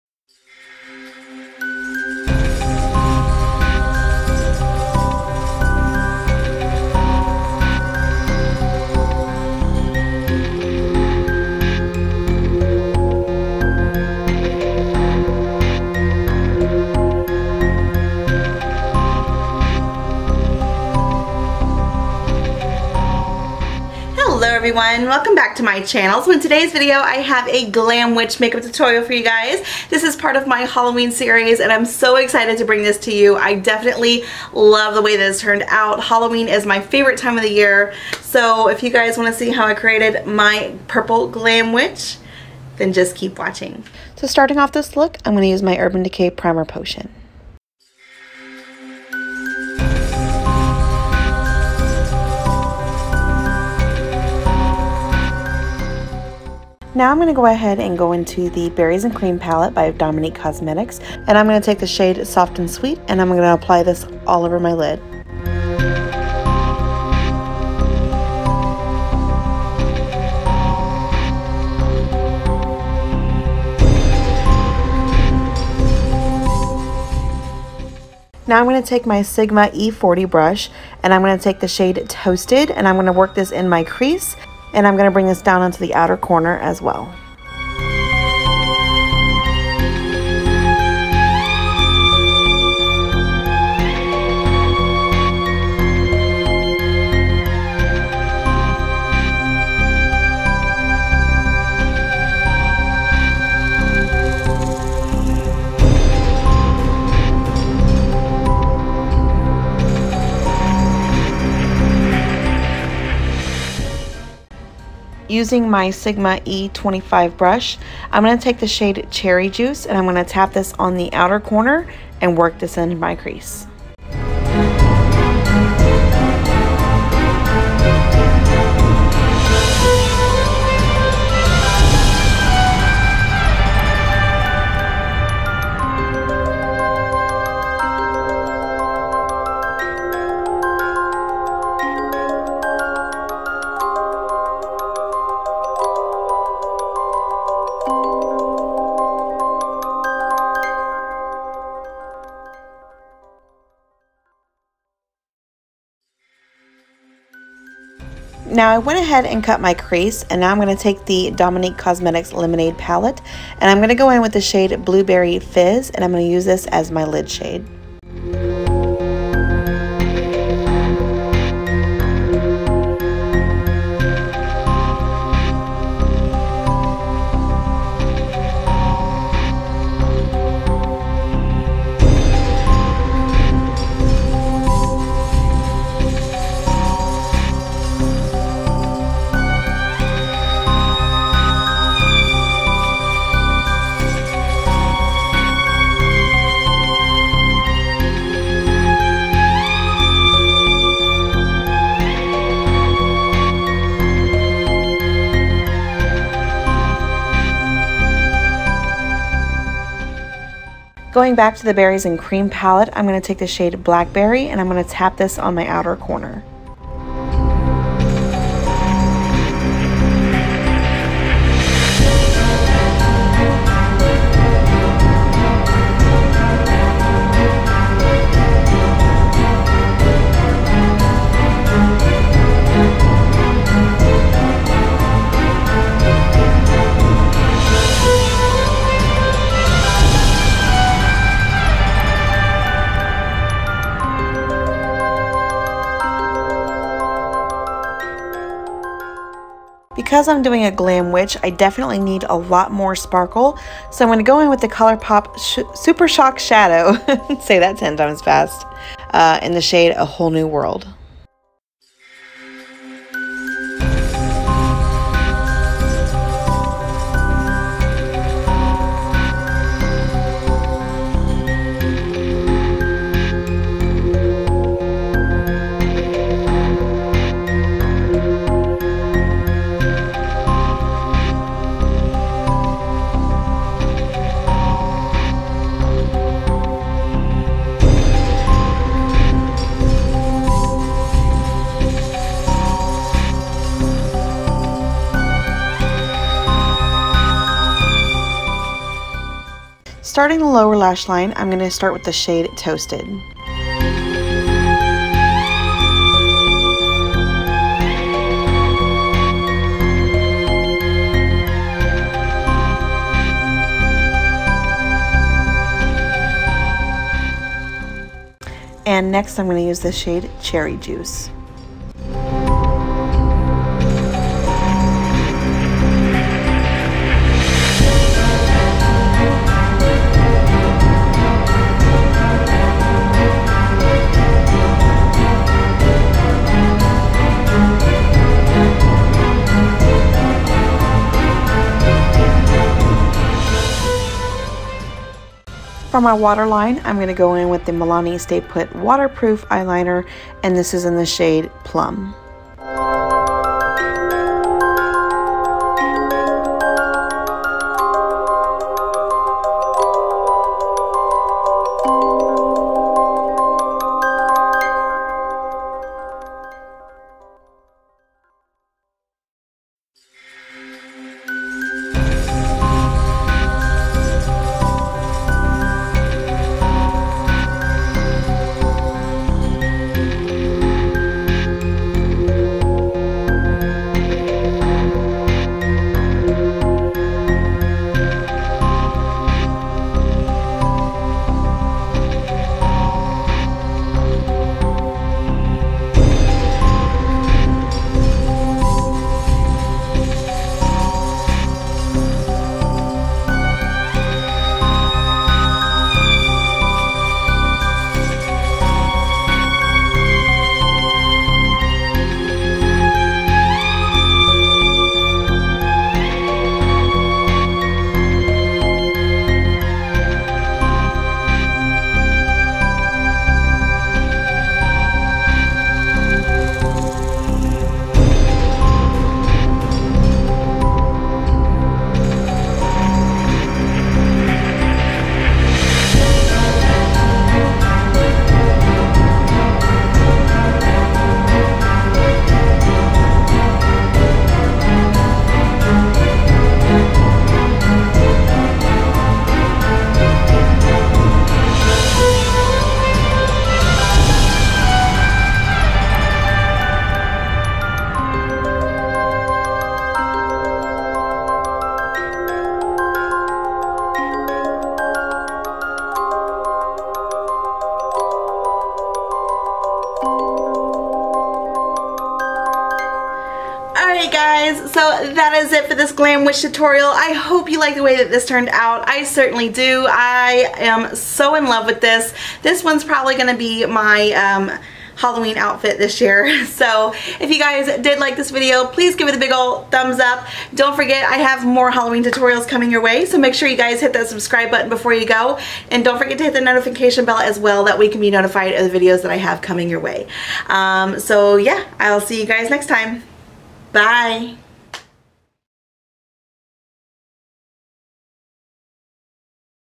Glam Witch Halloween Makeup Tutorial
Hey everyone! In today's video I bring to you a glam witch makeup tutorial!! I love how this turned out! I hope you guys do as well! Are you dressing up for Halloween this year?

Products Used:
DOMINIQUE COSMETICS Berries & Cream
DOMINIQUE COSMETICS Lemonade Palette
Colourpop Super Shock Shadow
ABH Berry Adore Blush Trio
Becca Amethyst Highlighter
Wet N Wild Scorpio Lip Gloss

Used the code CASSIE to save!!

If you shop online try Rakuten! You get FREE cash back!!!